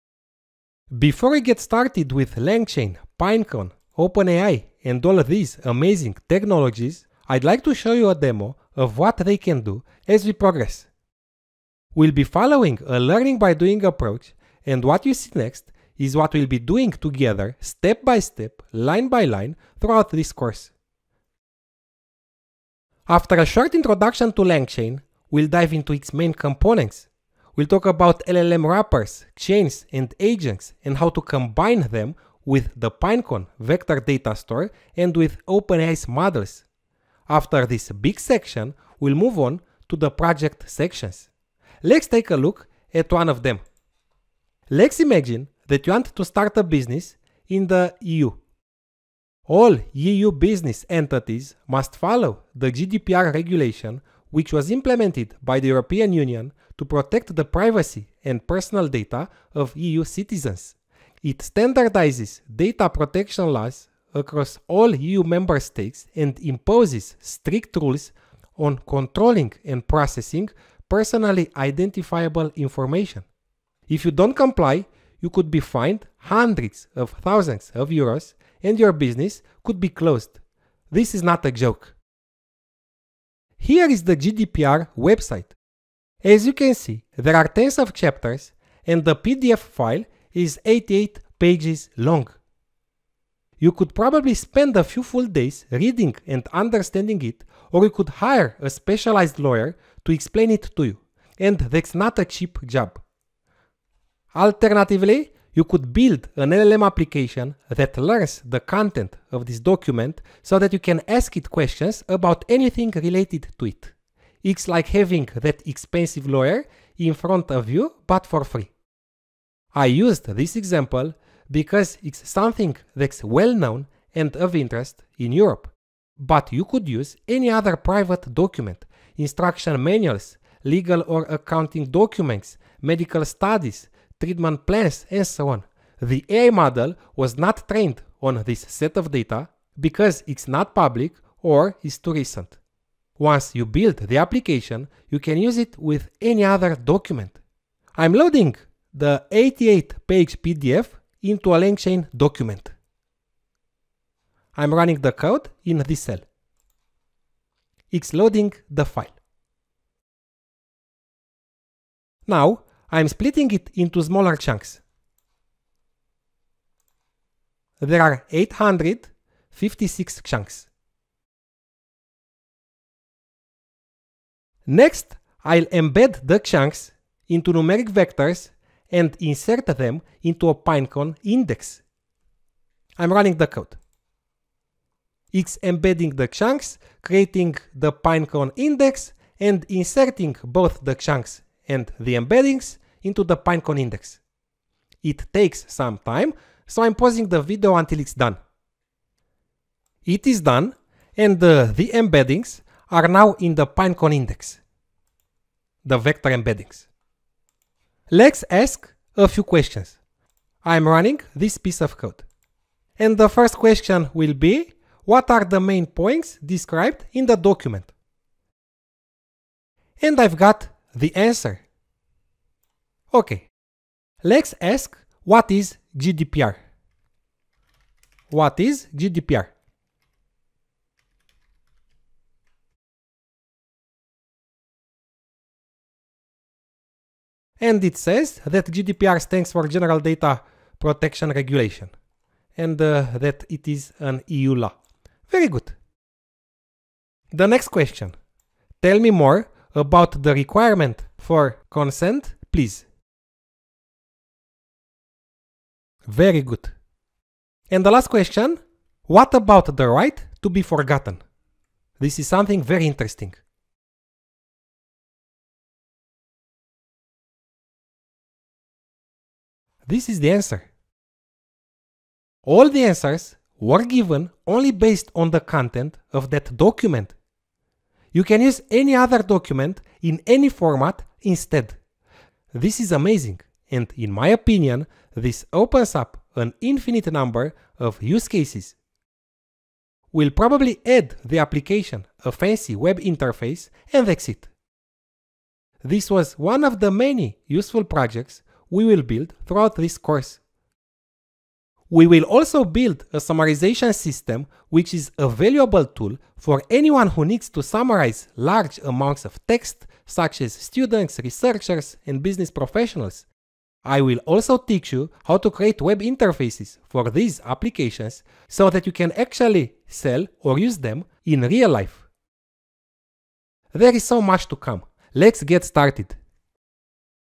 Demo: LangChain, Pinecone, and OpenAI
Take a look at my new course on LangChain.

LangChain connects LLMs with external sources of data and computation. It allows you to build LLM-powered applications easily.